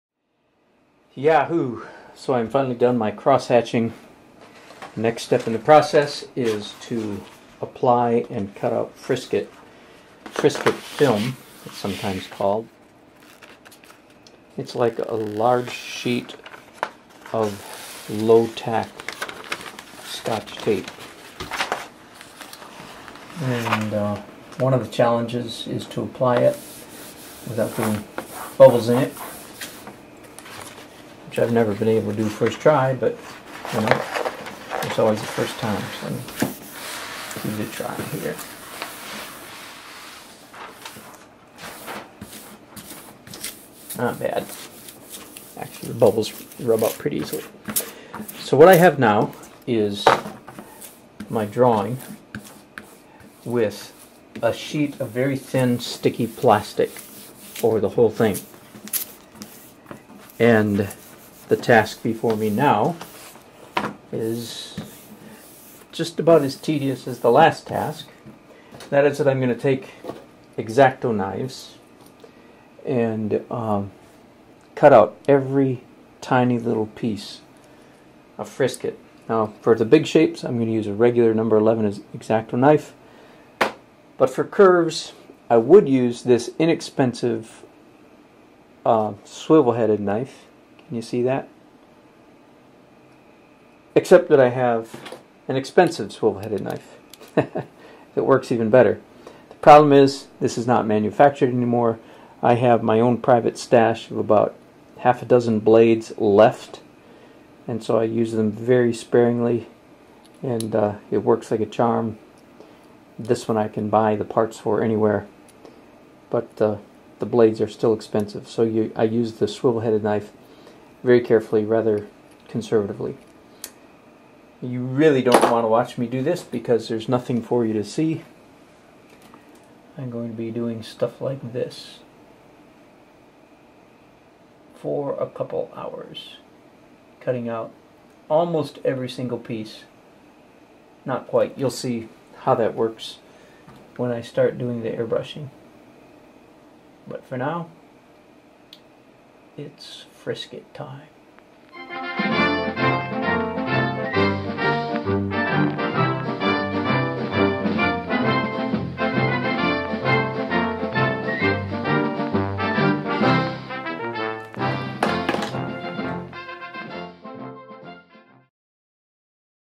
Step 5- Cutting the Frisket
This is Frisket film! So THAT'S how airbrush artists get those sharp, crisp edges!